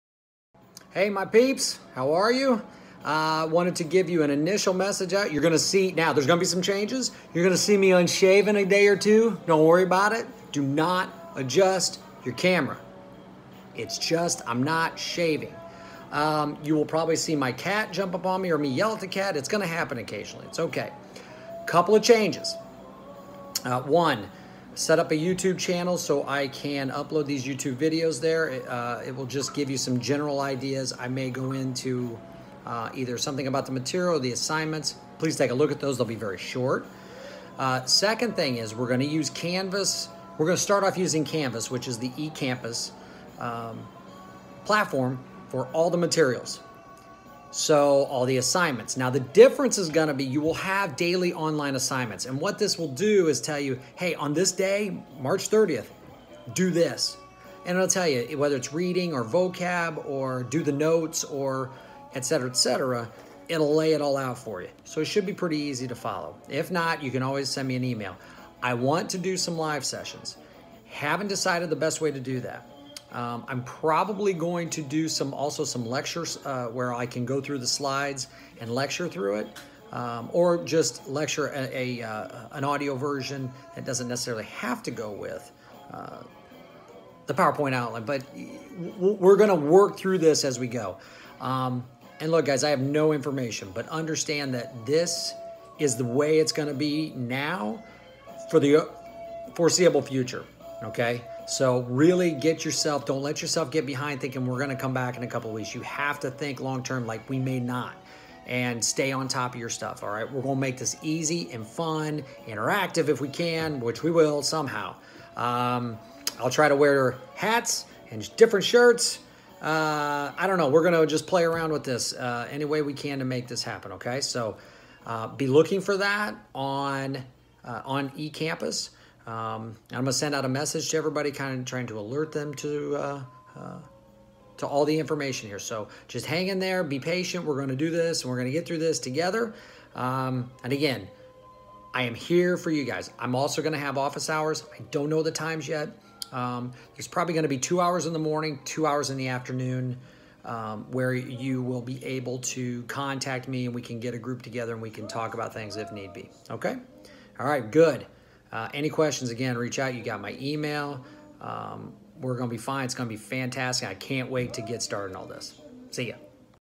Opening Communication for Coronavirus
Saying Hi to all my peeps at Lake Brantley.